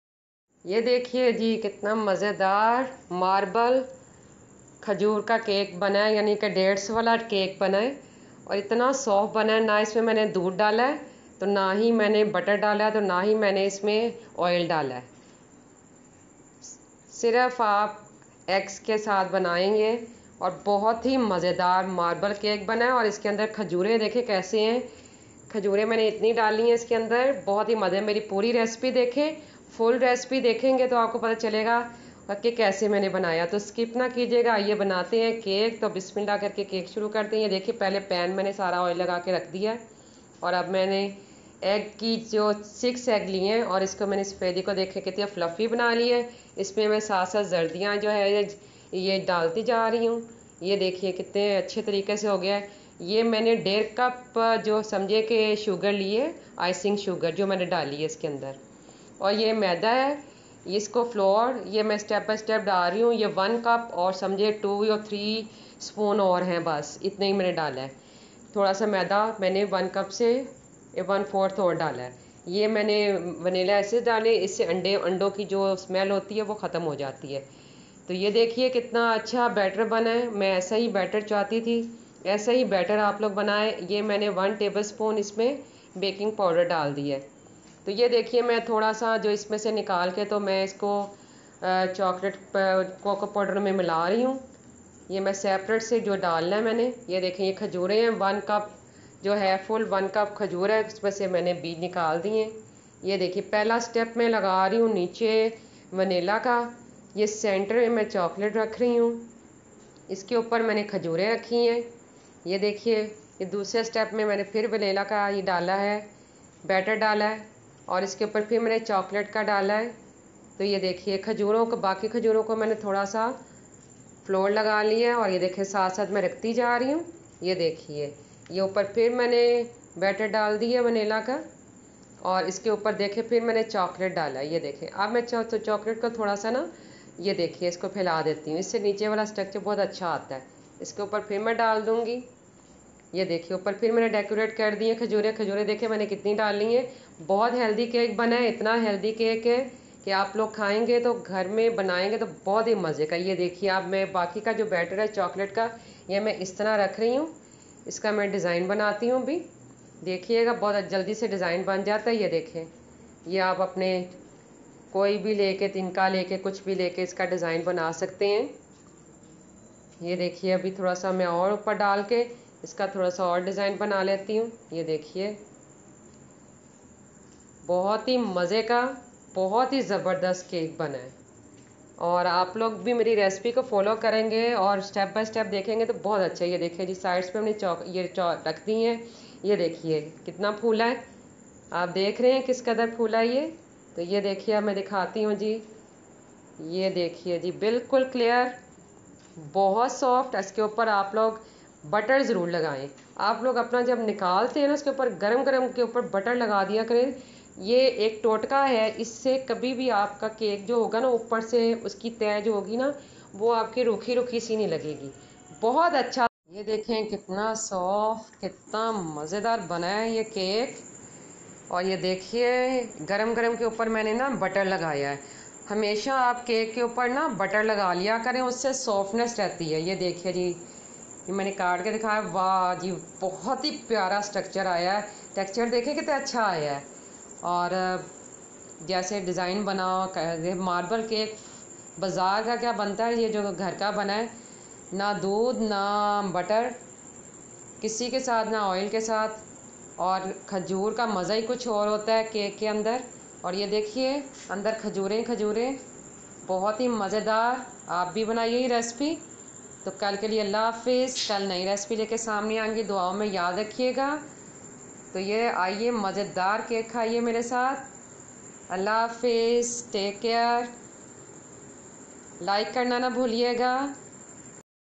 Bazar ke cake ko bhul jaaye ghar mein banae dates marble cake healthy cake by taste the best kitchen
Taste the best kitchen
Ingredients
11/4 cup flour
6 eggs
1 1/2 cup sugar
2tablespoon coco powder

almond, apricot,dates cake

watch all cakes recipe